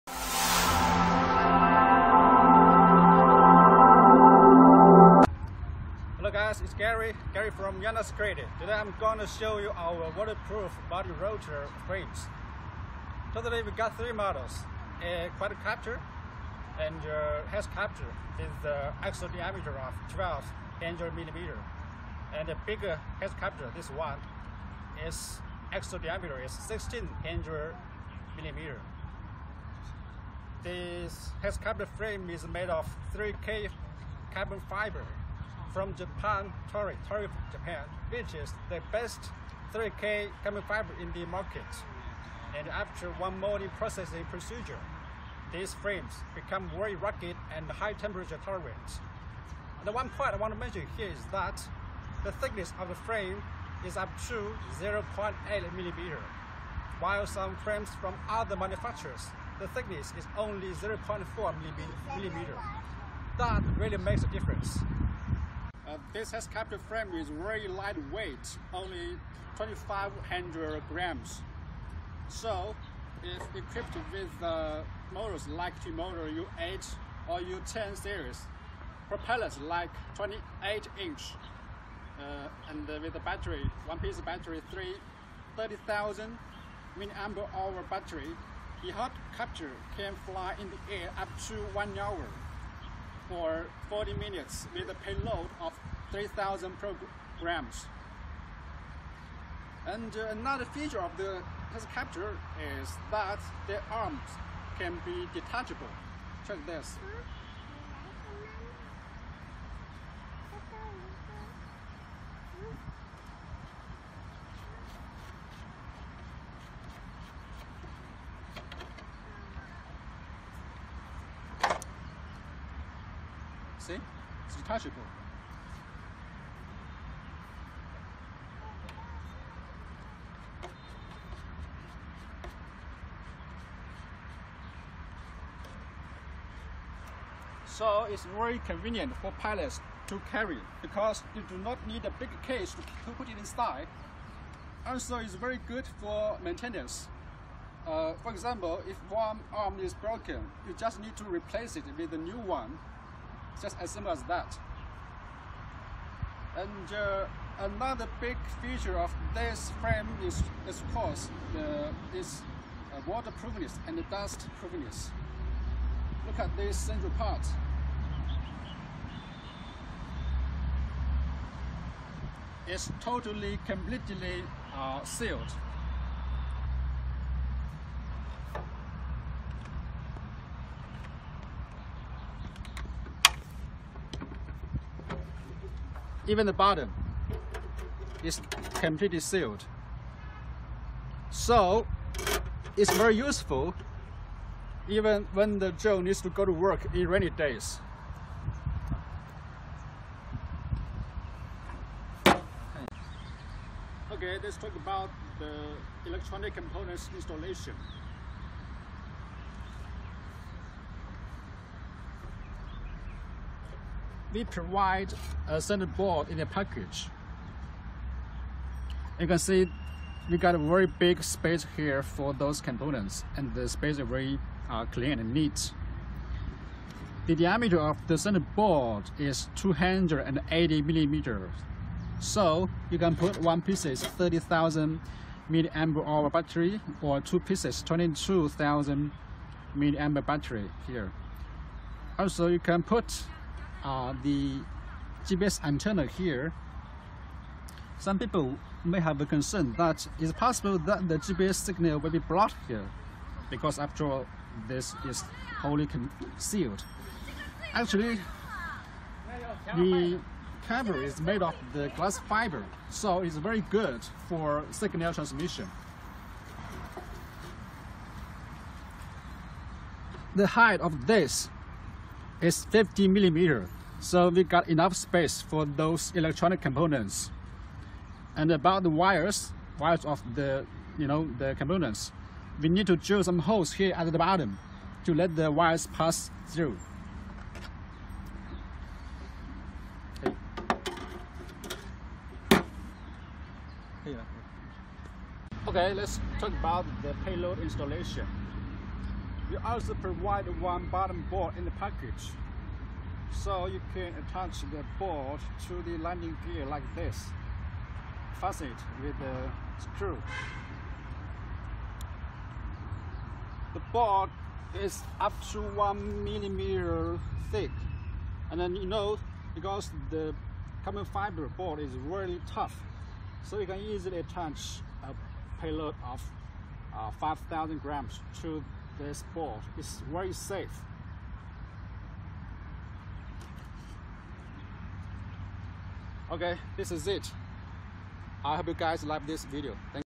Long flight time waterproof hexacopter frame review
The hexacopter frame YD6-1600S comes with many great features.
Its one-time forming carbon fiber reduces weight while still keep the stiffness,which makes it stronger and lighter for long time(up to 60 minutes) flying tasks.
For instance,equipped with a small camera like Gopro,the firefighter is able to monitor the fire scene for a long time which is crucial for the fire fighting.
Also you might use it for 3D mapping and other long time aerial photography tasks.

Furthermore,with a glass fiber canopy,the GPS signal will not be blocked.Its detachable frame arm makes YD6-1600S very easy to carry and easy for maintenance.

Another very useful feature is its waterproofness and dustproofness,which enable the drone to fly under most weather conditions,like rainy days.In this case,
it is an idea tool for police security monitoring,fire fighting,emergency research and rescue.

Features:
1)6 axis,1600mm axle diameter
2)12KG payload capability
3)60 minutes flight time(no payload)
4)Frame weight:2.5KG
5)Motor capatible:T-Motor U8,U10 series
6)Max propeller size:28 inch
7)Battery compatible:1pcs 22.2V 30000mah or 2pcs 22000mah lipo battery
8)Completely waterproof and dustproof